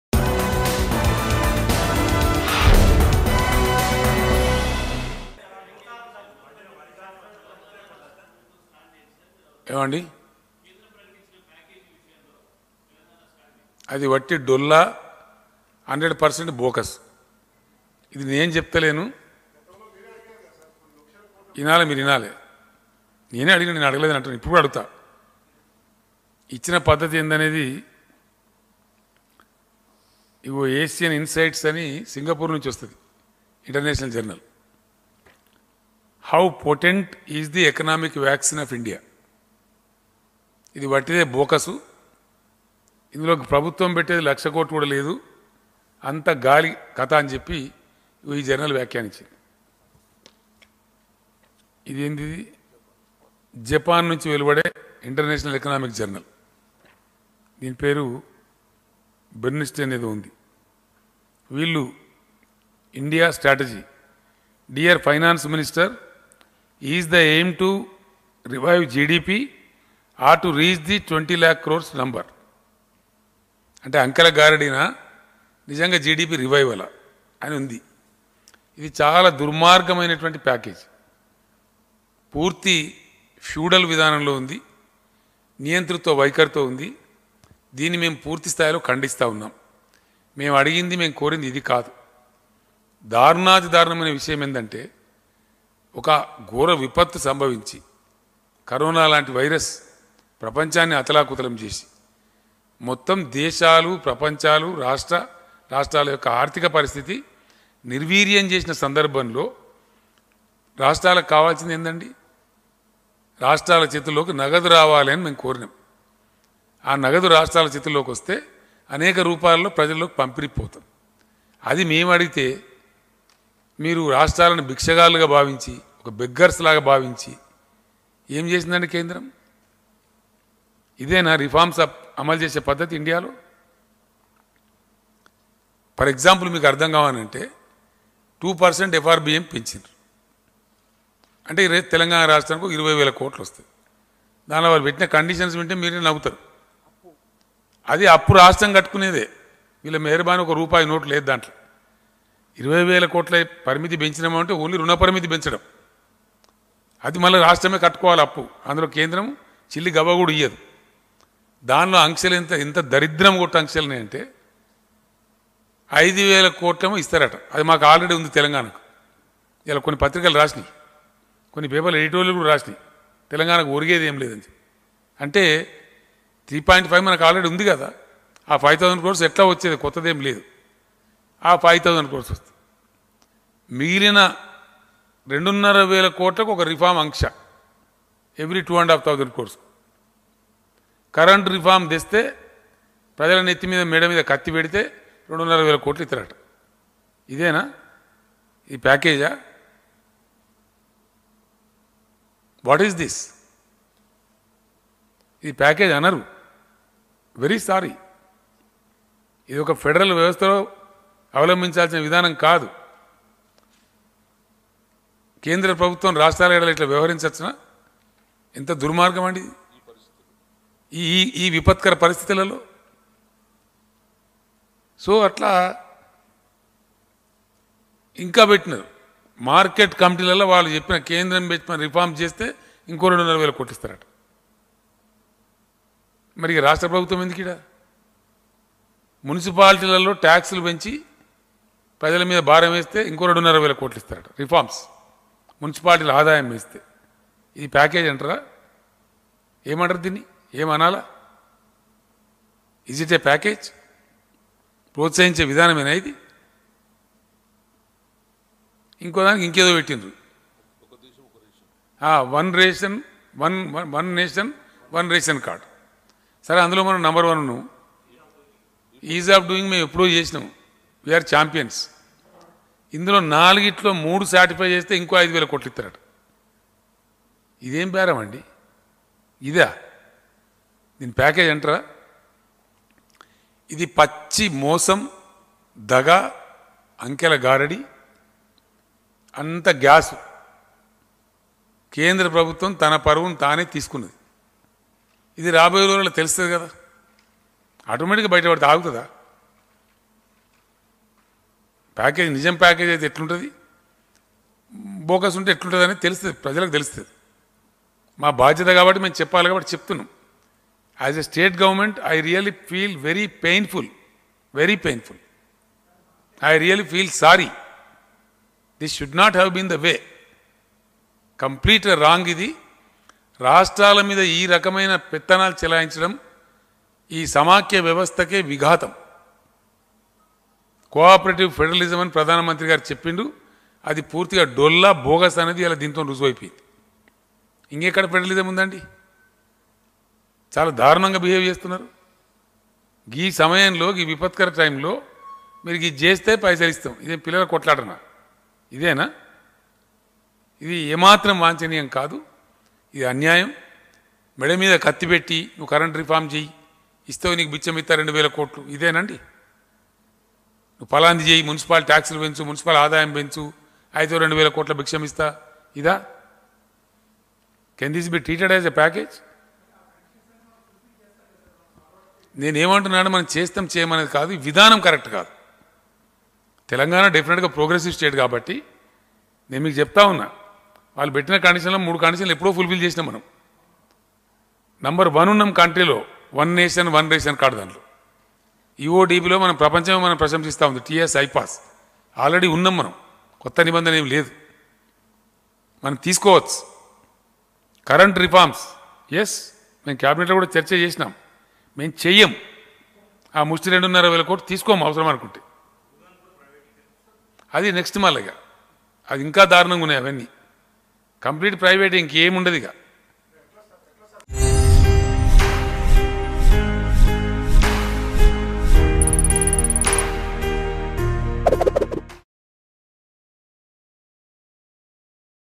CM KCR Speaks About PM Modi's 20 Lakh Crores Of Economic Package | Lockdown Updates | Mango News
CM KCR Speaks About PM Modi's 20 Lakh Crores Of Economic Package.